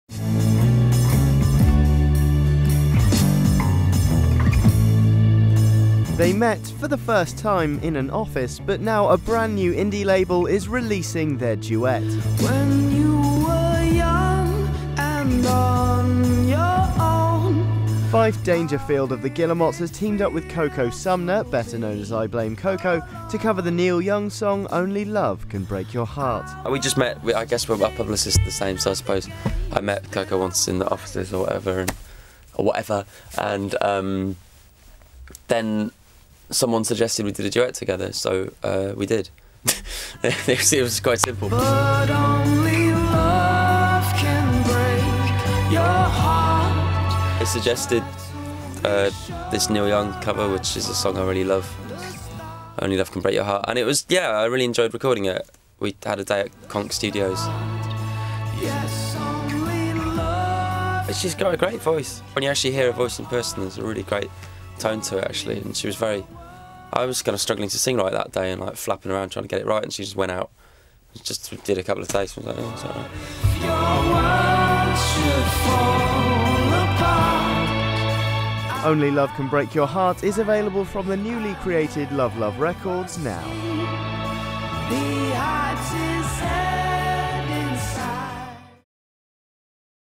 Fyfe Dangerfield talks singing with Coco Sumner
Fyfe Dangerfield of the Guillemots talks us through the experience of covering a Neil Young track with I Blame Coco.